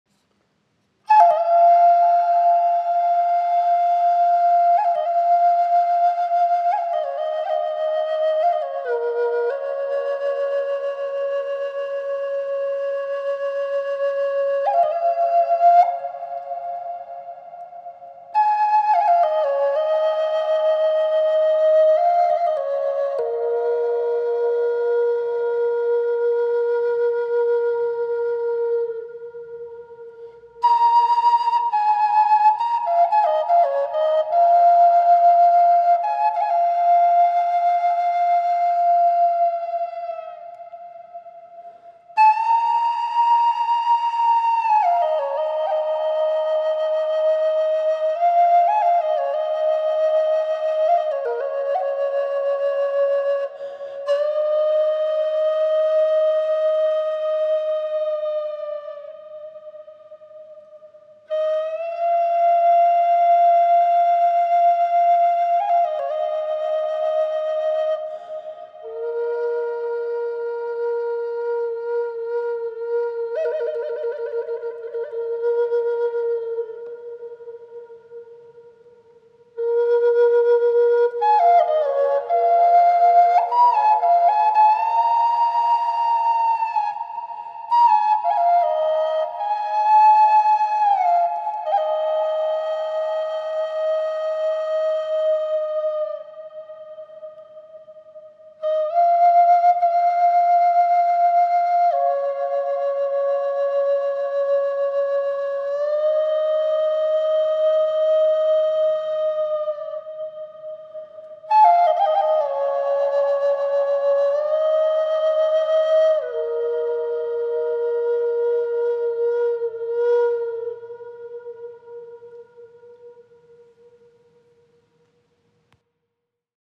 Native Style Branch Flute ~ made from chestnut tree ~ "B" 432hz ~ from Many Moons Flutes
Native American Style Branch Flute in "B" 432 hz. From Many Moons Flutes.
Insta @manymoonsflutes for more content